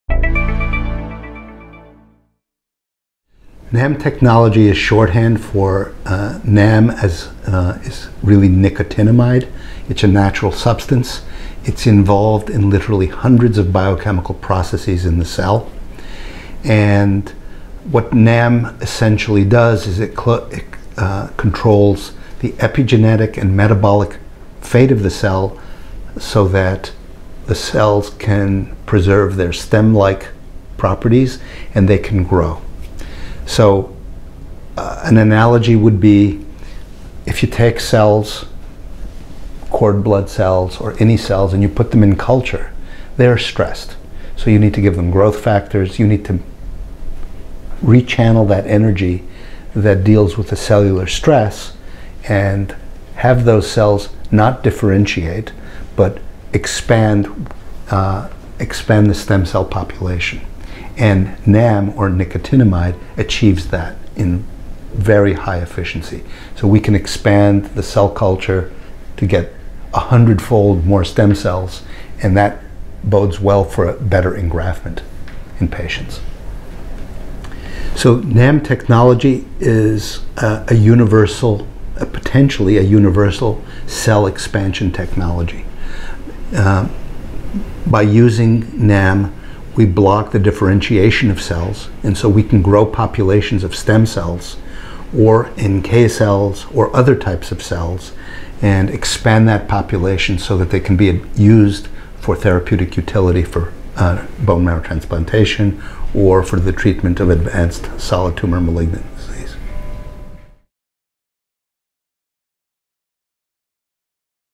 Gamida Cell's NAM Technology
Julian Adams discusses Gamida Cell's NAM technology to develop two of its lead clinical programs by expanding blood forming stem and progenitor cells from umbilical cord blood to create highly functional, stand-alone grafts that serve as a universal bone marrow transplant solution.

Gamida Cell has successfully harnessed its NAM platform technology to leverage the advantages of cord blood and overcome its limitations by increasing the number of CD34+ stem and progenitor cells, while enhancing their homing and engraftment efficiency.